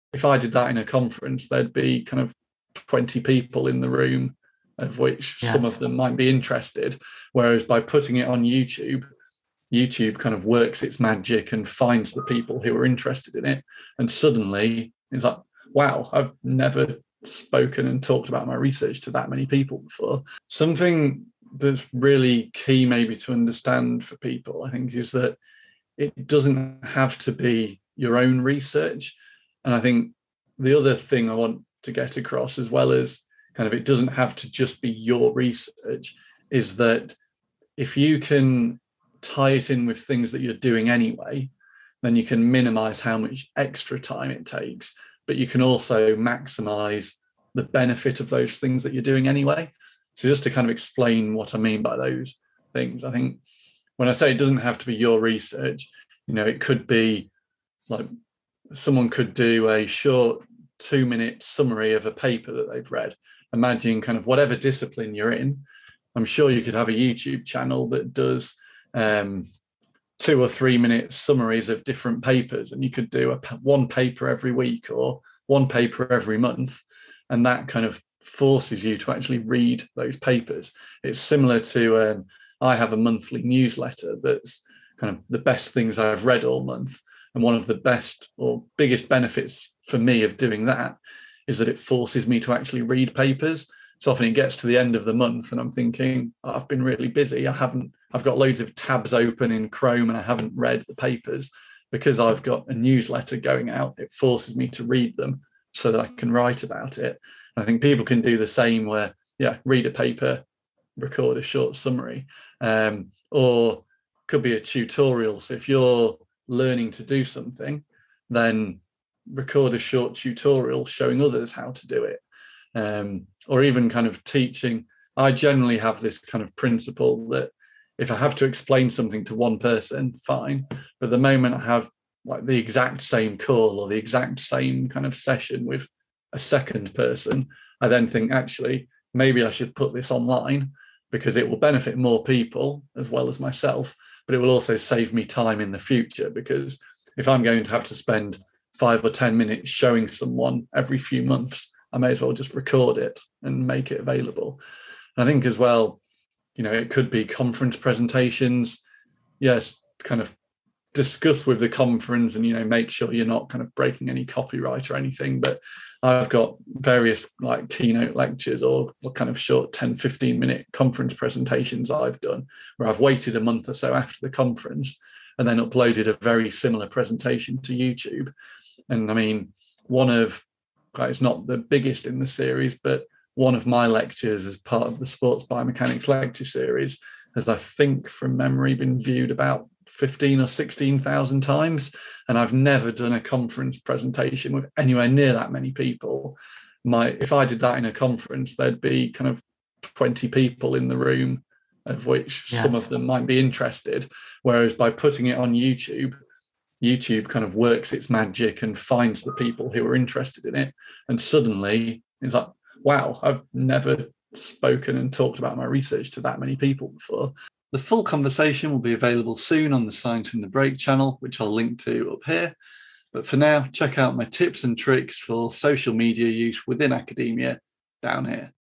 YouTube for Academia – INTERVIEW | Science in the Break
How to start an academic YouTube channel in 2022 - what should you post? How can you include YouTube and video creation in your schedule alongside research and teaching? How can you use YouTube to promote your research and publications, and work alongside journals and conferences?

This video is part of an interview by Science in the Break, in which we discuss these topics at length